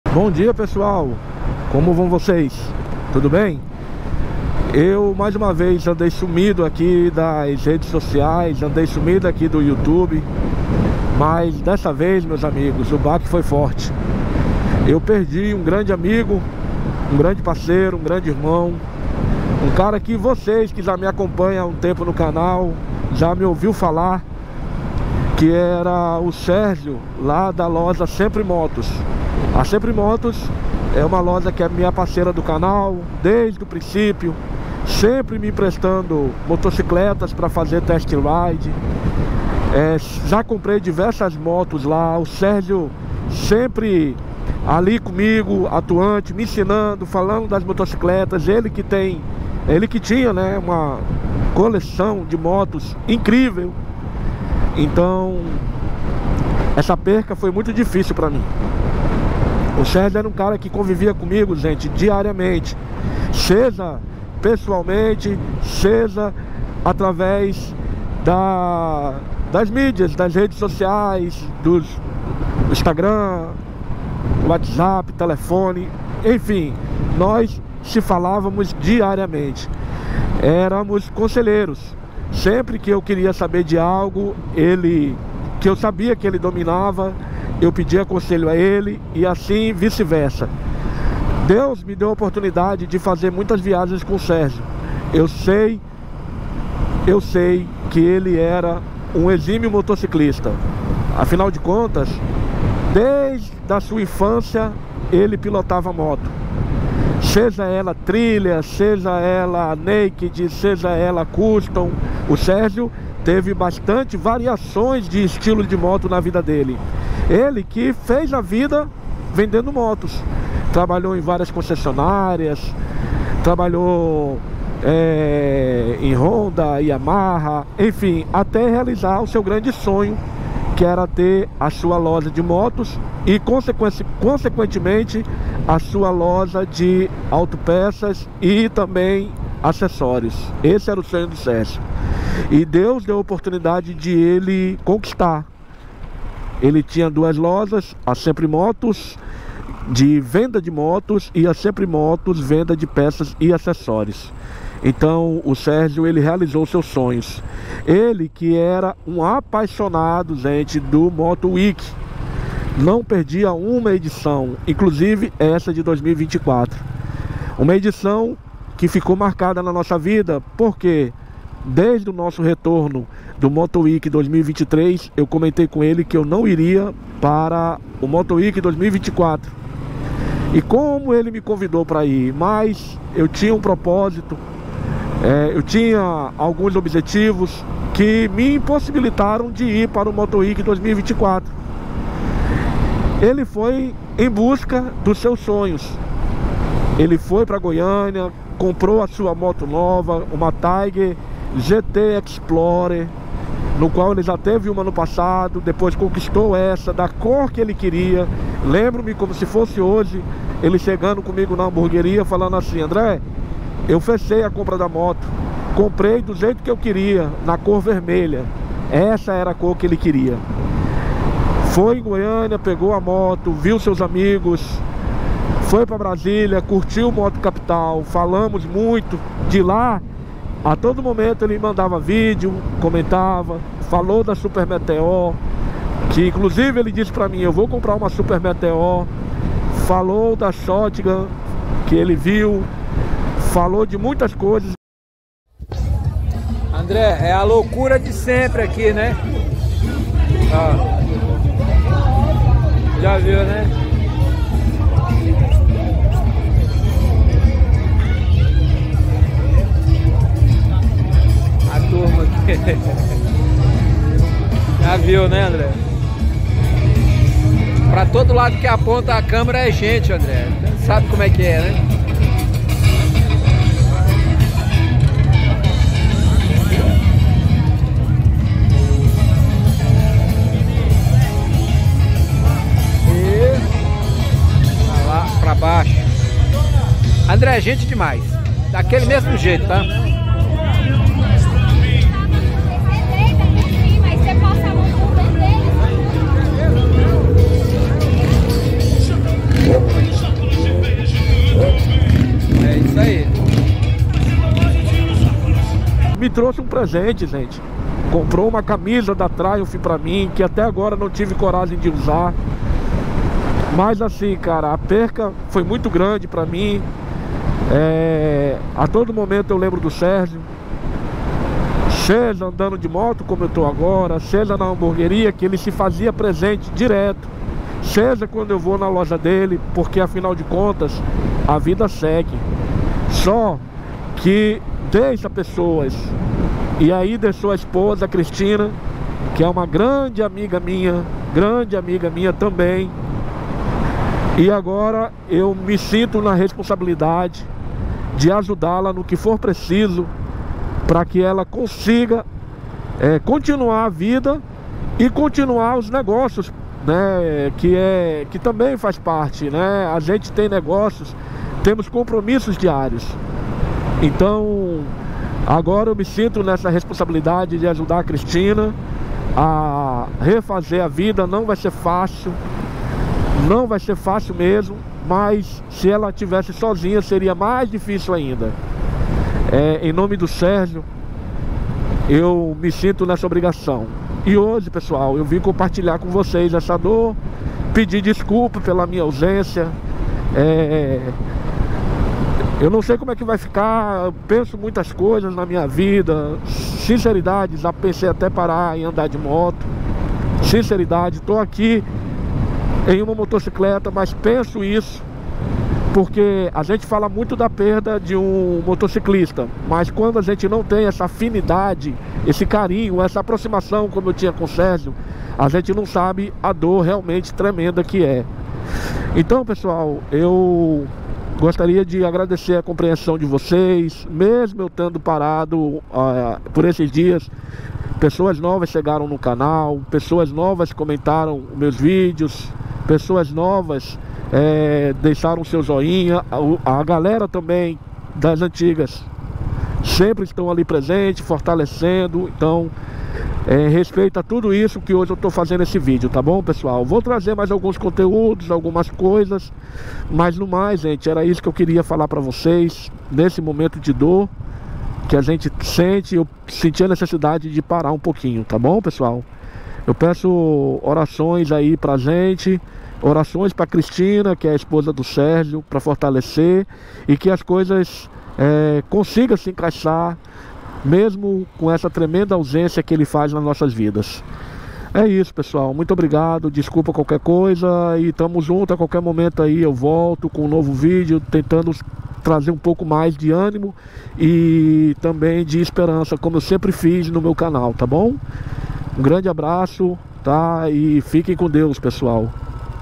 Luto por um irmão
A dor de perder alguém próximo é muito grande, apenas quando isso acontece sabemos de fato o tamanho da dor. recente perdi um irmão, grande amigo que esteve comigo em diversos momentos da minha vida, onde tivemos o privilégio de viajar juntos e compartilhar histórias em duas rodas. o Sérgio era uma pessoa querida e sentiremos por muito tempo a sua falta.